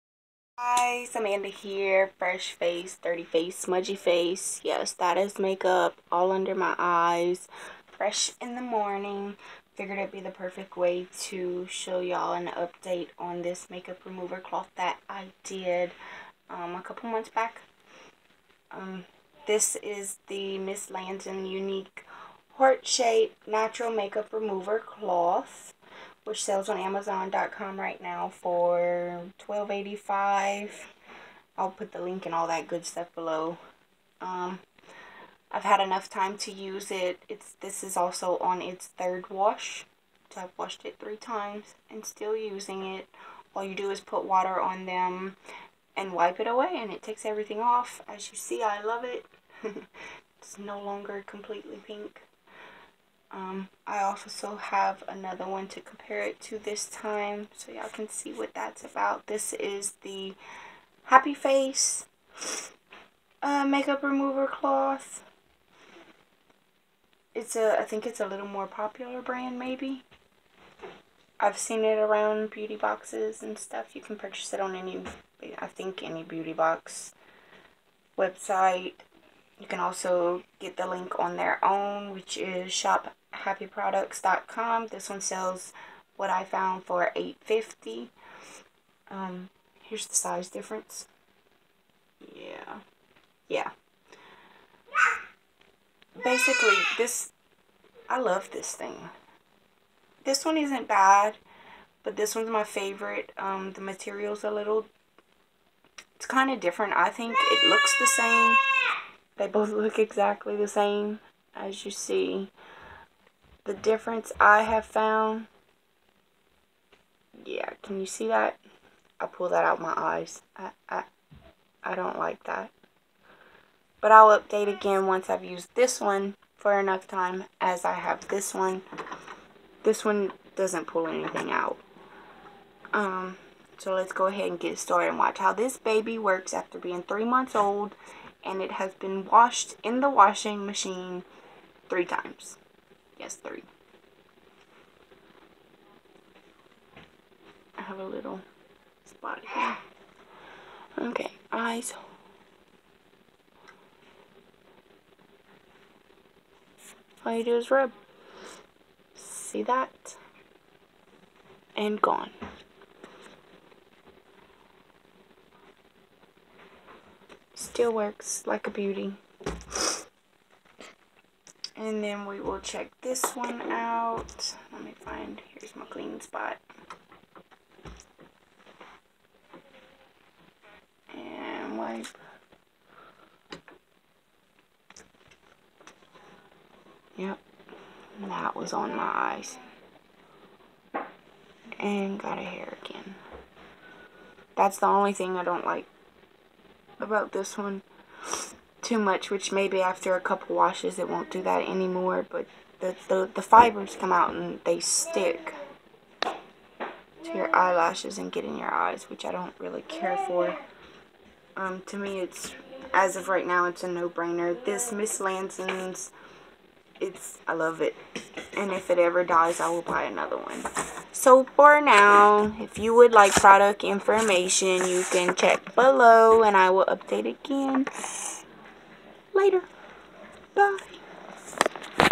Get you some update! Miss Landson vs HappyFace Make-Up Remover Cloths
Hey Ya'll!
Its been 5 months since my first use of the Miss Landson Natural Make-up remover cloth so I figured it was update time! Vie also included a new wipe, the HappyFace cloth.

✨Miss Landson Product Link:
$12.85

✨HappyFace Product Link:
$8.50

You can also check out my blog for moreFAB-u-LOUS finds and get my P.O.V.

Thanks!
💋Muah!